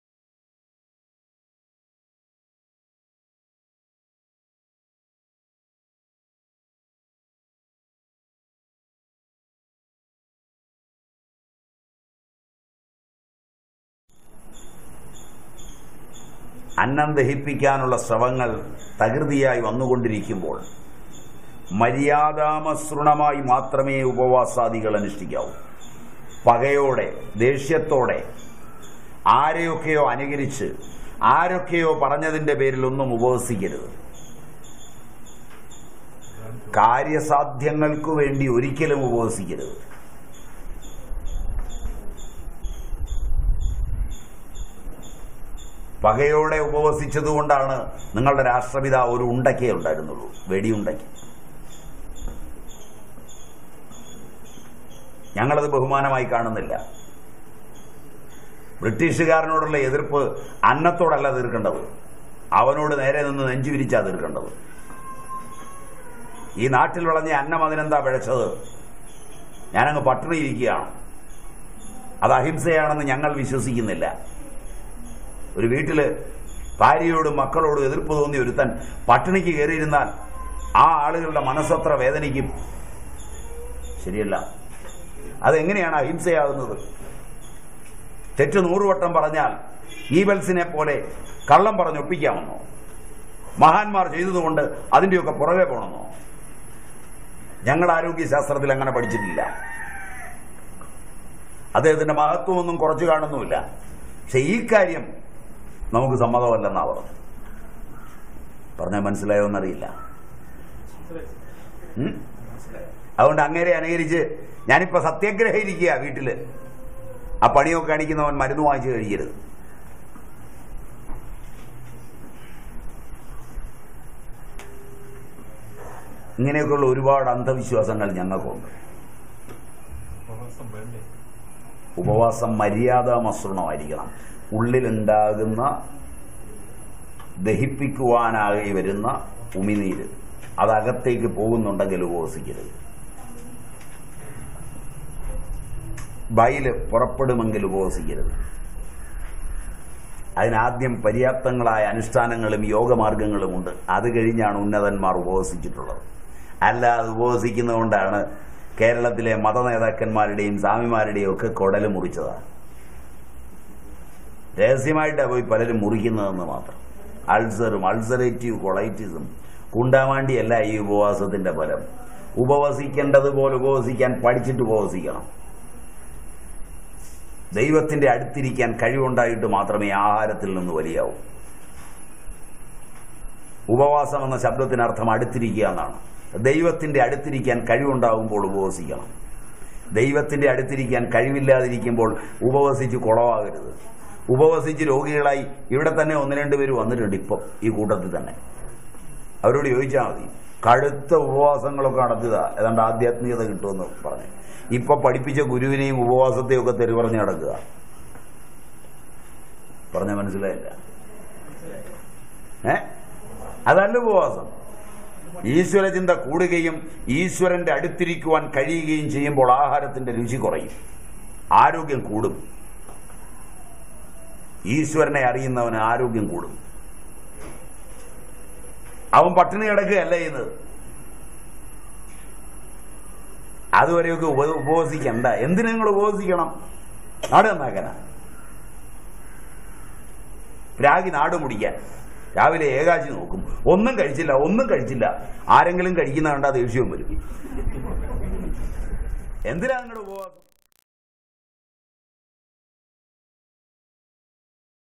SWAMI NIRMALANANDA GIRI | UPAVASAM | ഉപവാസം
എങ്ങനെ ഉപവസിക്കണം?
എപ്പോള്‍ ഉപവസിക്കണം?
എന്താണ് ഉപവാസം?
ഉപവസിക്കാന്‍ വേണ്ട മുന്‍കരുതലുകള്‍ എന്തൊക്കെ?
സ്വാമിജി വിശദീകരിക്കുന്നു.